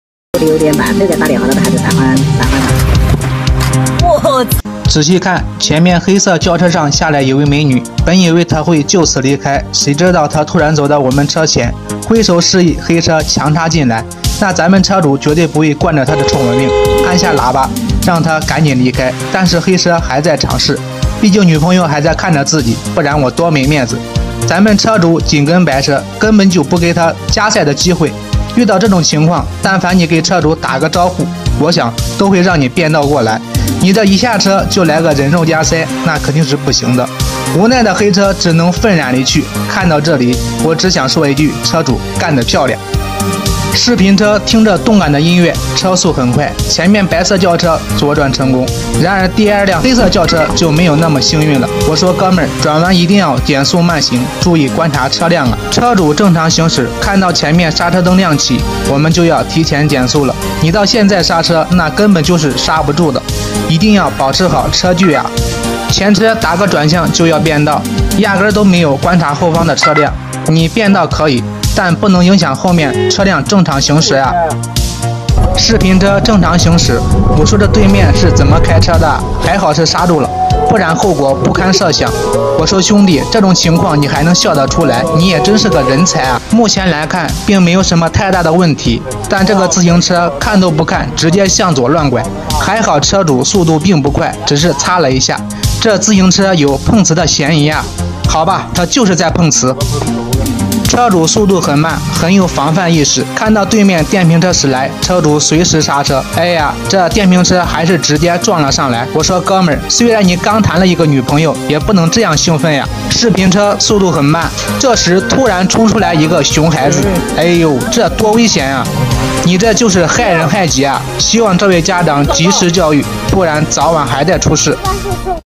美女人肉加塞，车主干的漂亮
道路千万条，安全第一条。